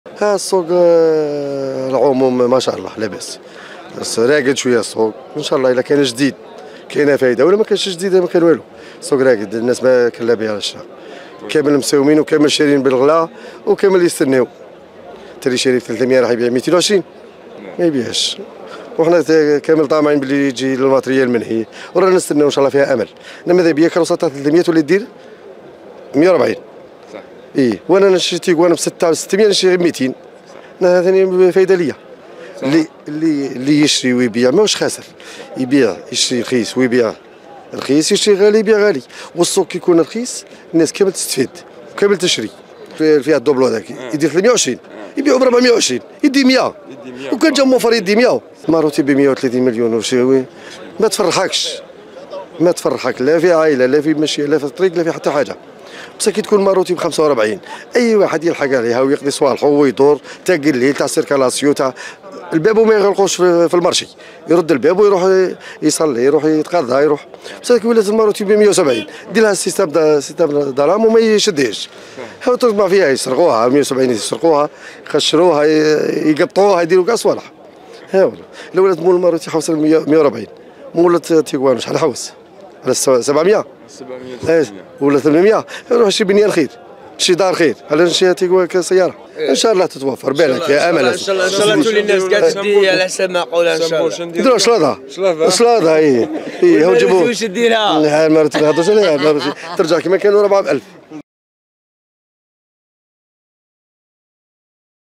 تيبازة شااهد..تصريح مواطن بخصوص أسعار السيارات المستعملة بحجوط..السوق طاح ت
تيبازة شااهد .. تصريح مواطن بخصوص أسعار السيارات المستعملة بحجوط ... السوق طاح تتوفر الفيات السيمبول ديروها سلاطة